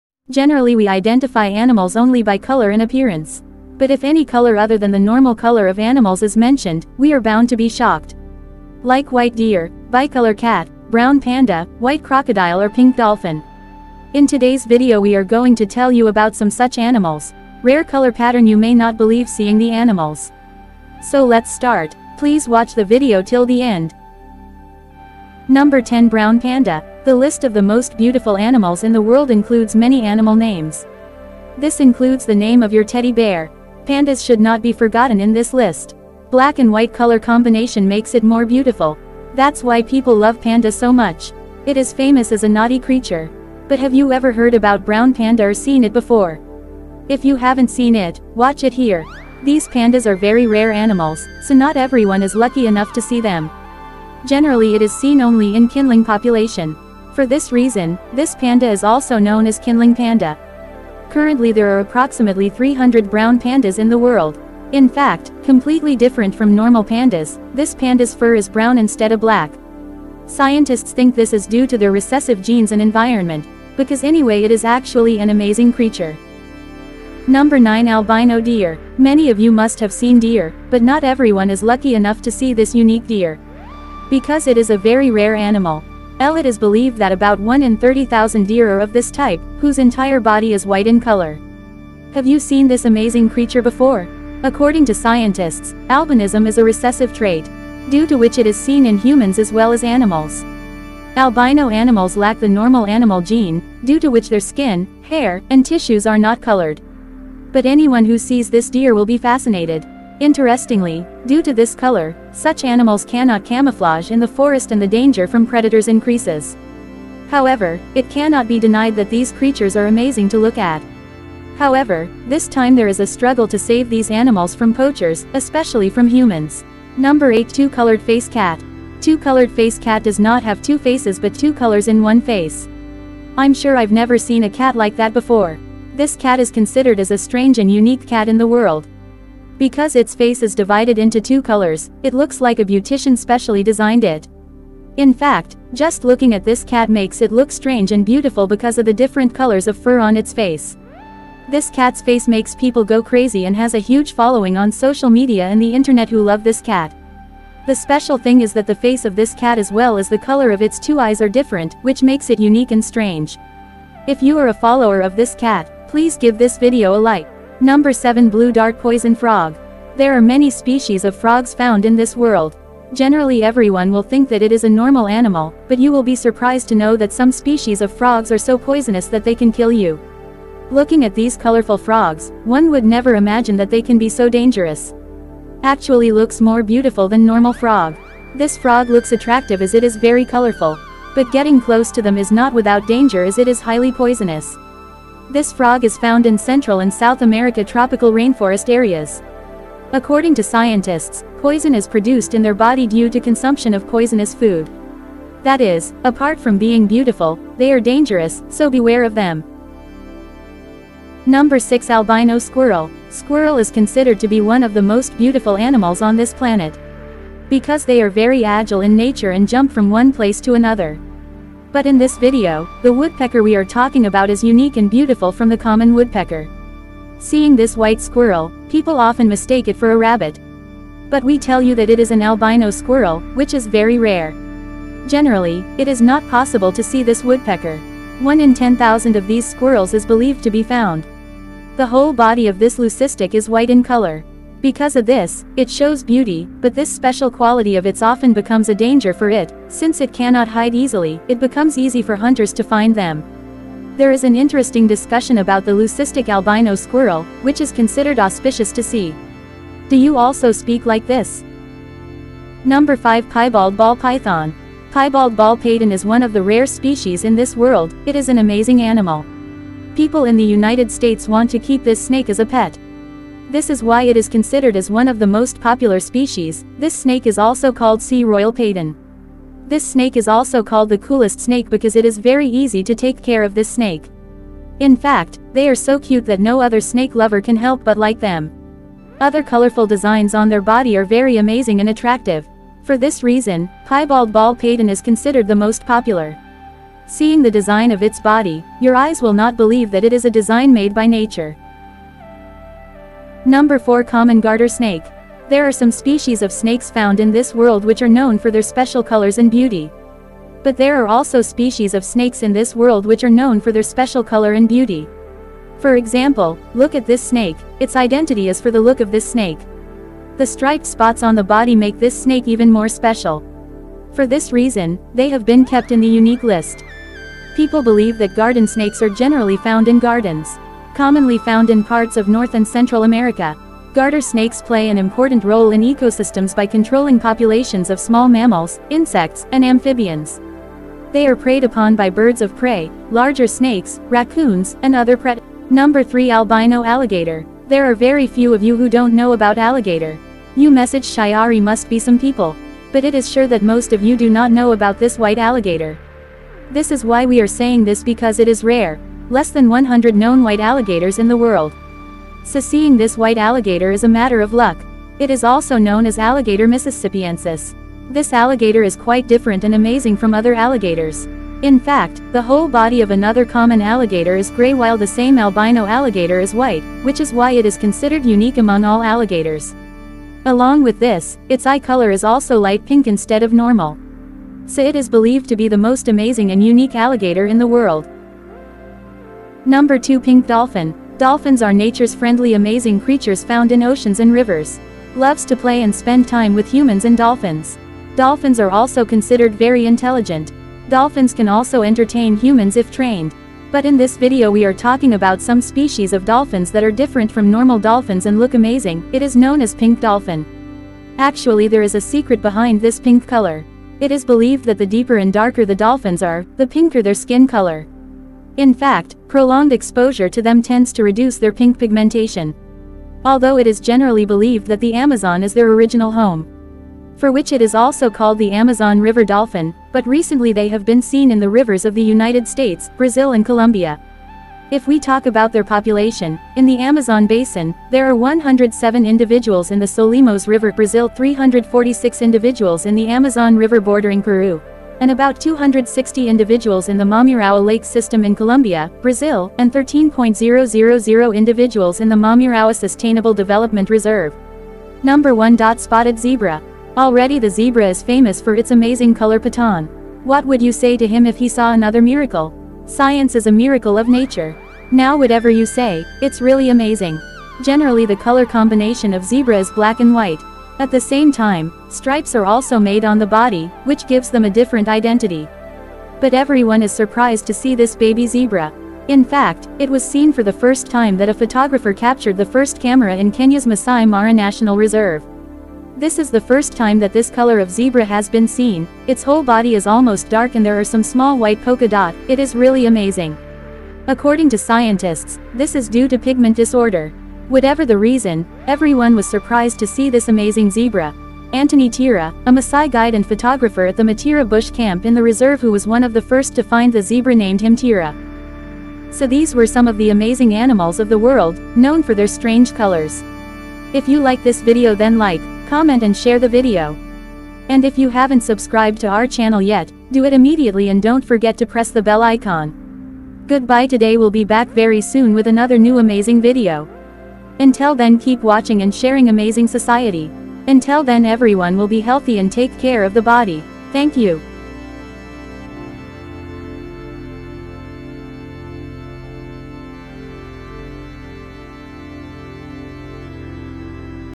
Creatures With The Most Unique And Rare Colors | Unique Animals With Rare Color Patterns
Generally we identify animals only by color and appearance.  But if any color other than the normal color of animals is mentioned, we are bound to be shocked.  Like white deer, bicolor cat, brown panda, white crocodile or pink dolphin.  In today's video we are going to tell you about some such animals.  rare color pattern You may not believe seeing the animals. So let's start. Please watch the video till the end.

Video Chapter:-
00:00 - Video Introduction.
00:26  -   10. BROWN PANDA
1:17  -  9. ALBINO DEER
2:07   - 8. TWO COLOURED FACE CAT
2:48  - 7. BLUE DART POISION FROG
3:31 -   6. ALBINO SQUIRREL
4:23  -  5. PIEBALD BALL PYTHON
5:03 -  4. COMMON GARTER SNAKE
5:46 - 3. ALBINO ALLIGATOR
6:30 -  2. PINK DOLPHIN
7:34 - 1. DOT SPOTTED ZEBRA
8:29 - Ending.